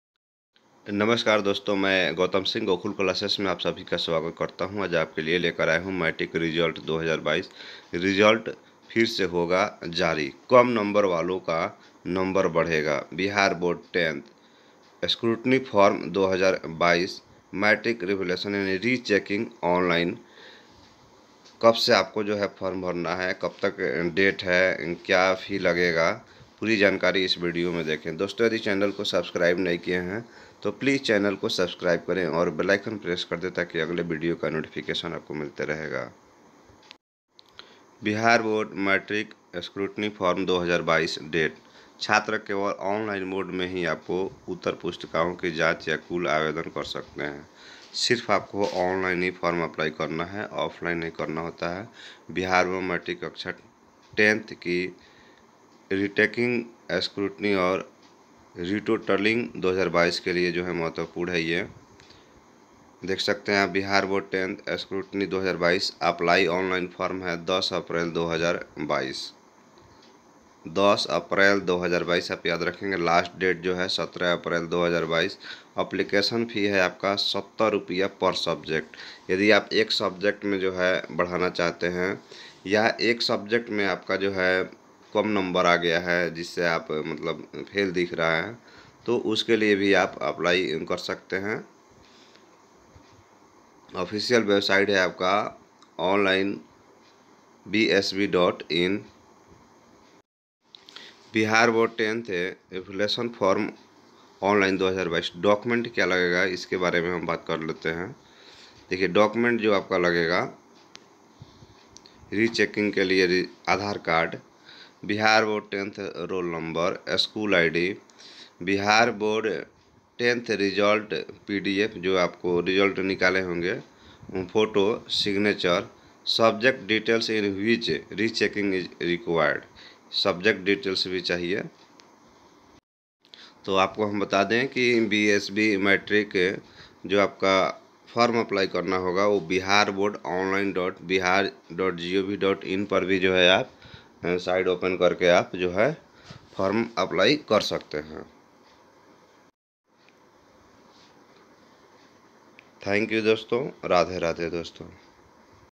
bihar board 10th Rechecking Online form 2022/Bihar board 10th Scrutiny Form 2022,Matric Revaluation
bihar board 10th Rechecking Online 2022 // Bihar board 10th Scrutiny Form 2022, Matric Revaluation

link